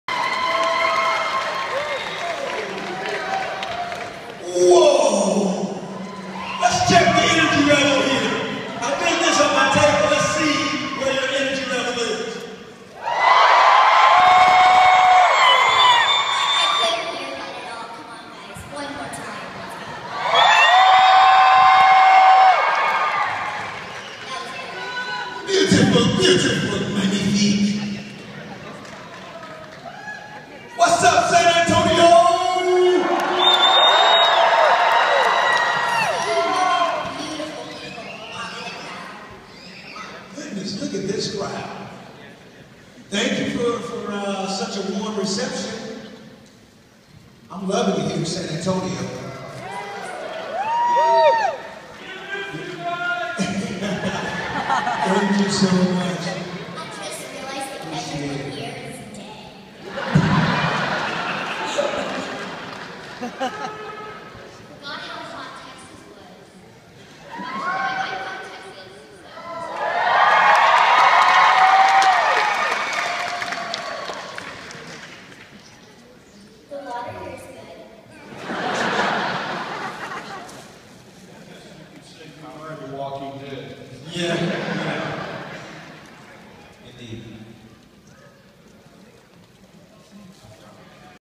The Walking Dead panel
Short video of a few cast members from TWD at Alamo City Comic Con 2014